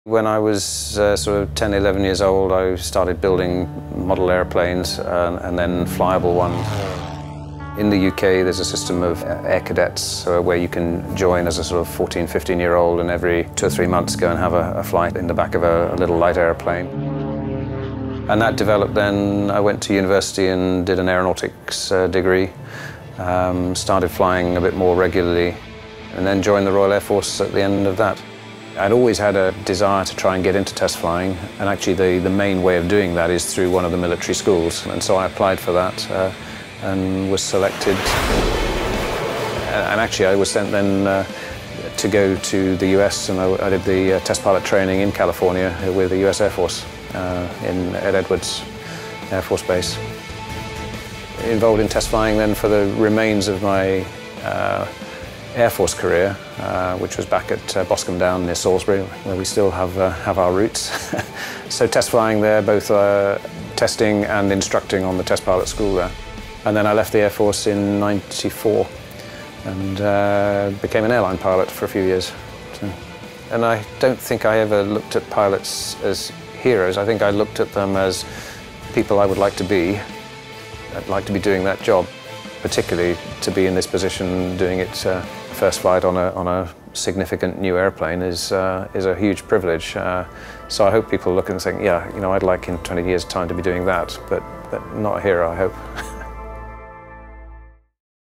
Peter Chandler: flying Airbus' pioneering aircraft
Since joining Airbus in 2000, Peter Chandler has been a flight test pilot for the company, and has been deeply involved with the development of its A350 XWB and A380.